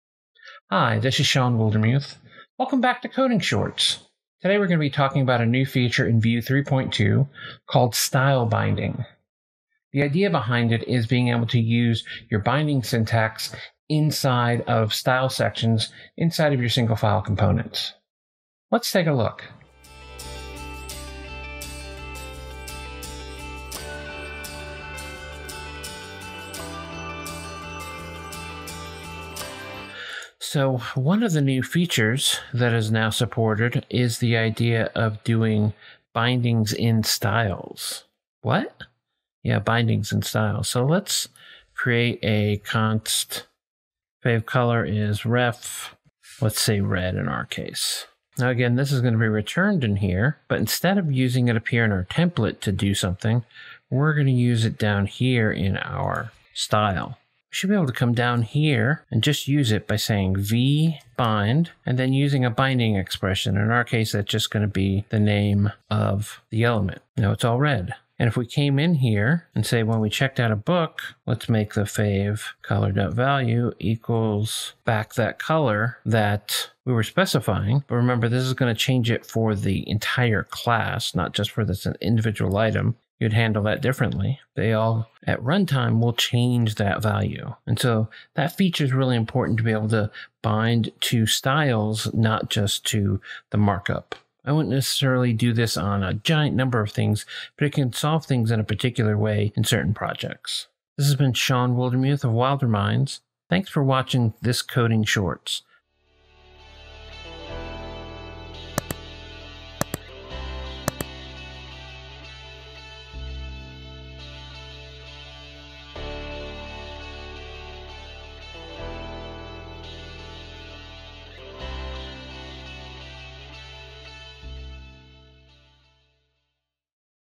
Coding Shorts: Vue 3.2 - Style Binding
Another Coding Short on Vue 3.2, this extra-short video shows you how Style Binding works in the newest version of Vue.

If you like this video, you might like other videos in my Instructional Videos: